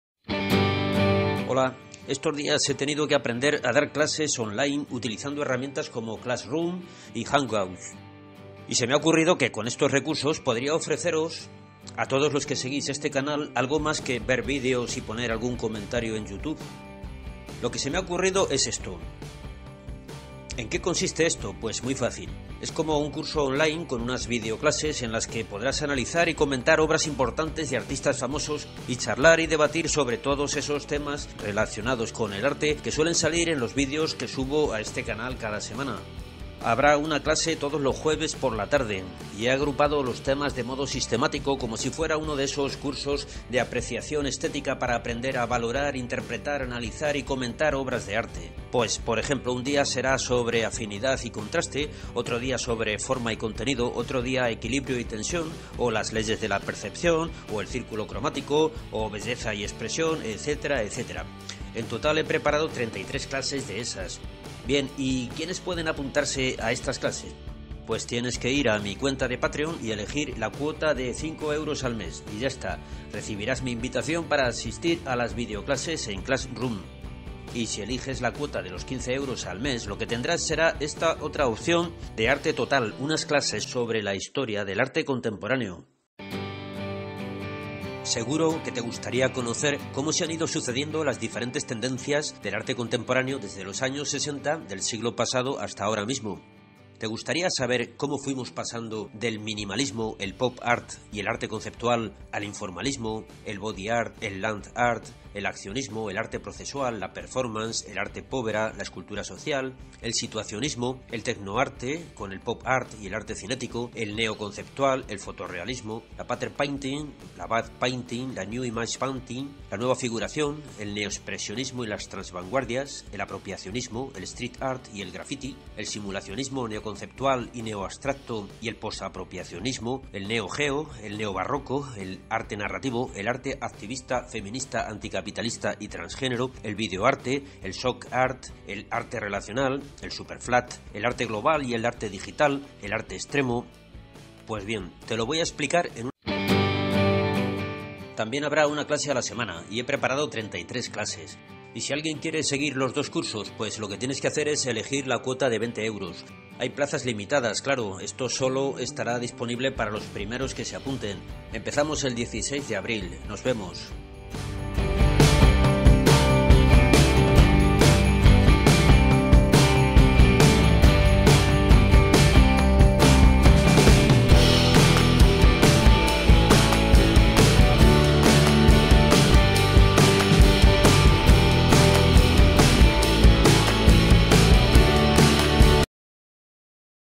Arte total.
Estos días he tenido que aprender a dar clases online utilizando herramientas como Classroom y Hangouts… Y se me ha ocurrido que con estos recursos podría ofreceros a todos los que seguís este canal algo más que ver vídeos y poner algún comentario en You Tube…
Curso de Arte TOTAL.